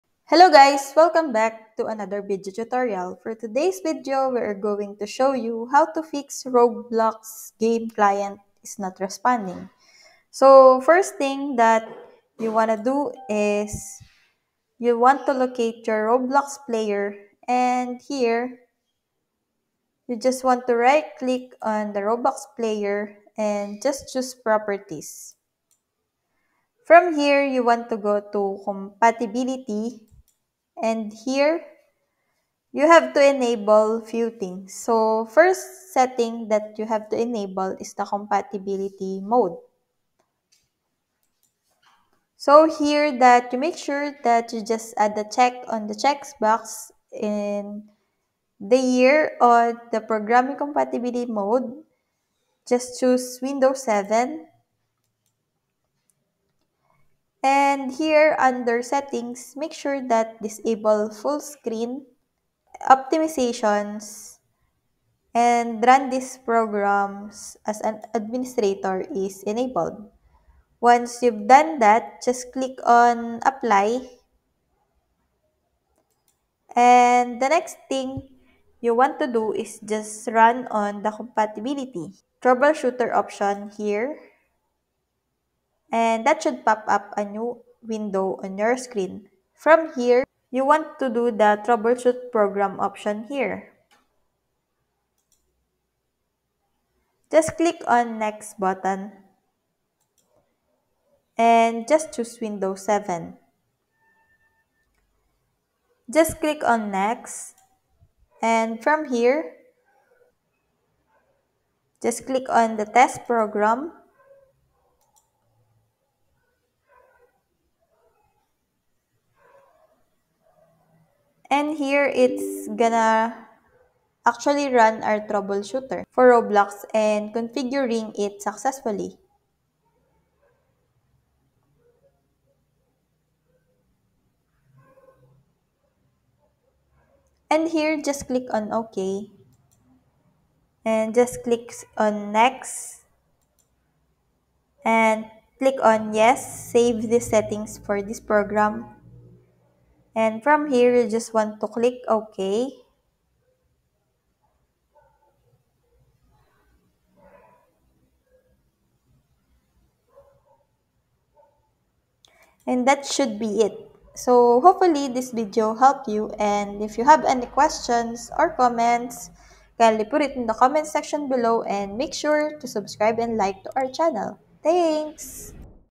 How To Fix Roblox Game Client Has Stopped Working 2023 | Full Guide
How To Fix Roblox Game Client Has Stopped Working 2023

In this tutorial video from Roblox, we explain to you the best way to Fix Roblox Game Client Has Stopped Working 2023.

💡Want to know something and this video is not yet on our channel?

📸 Our videos are created to help people with their problems online.